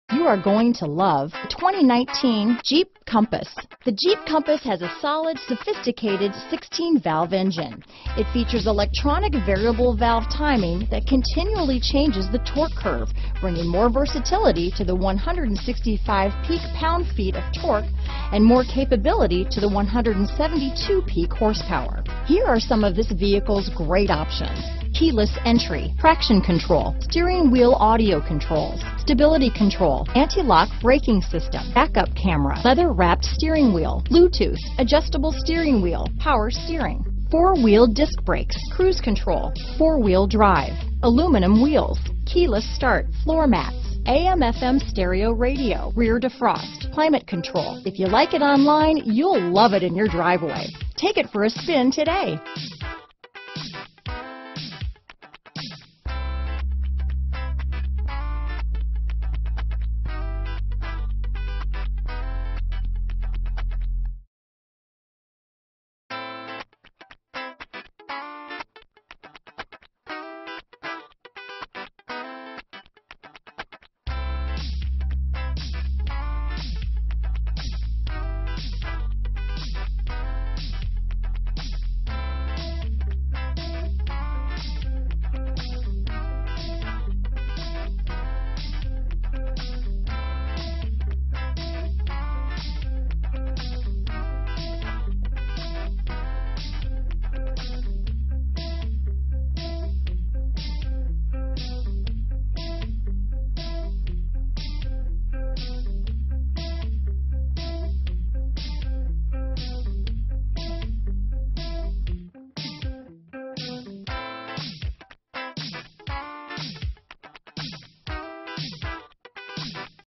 2019 Jeep Compass Mt. Airy, Westminster, Skysville, Germantown, Frederick, MD 33345
New 2019 Jeep Compass available in Mt. Airy, Maryland at Century Mt Airy. Servicing the Westminster, Skysville, Germantown, Frederick, MD area.
2019 Jeep Compass Latitude 4x4 - Stock#: 33345 - VIN#: 3C4NJDBB3KT786682

granite crystal metallic clearcoat 2019 Jeep Compass Latitude 4WD 9-Speed Automatic 2.4L I4&lt;br&gt;&lt;br&gt;Recent Arrival! 22/30 City/Highway MPG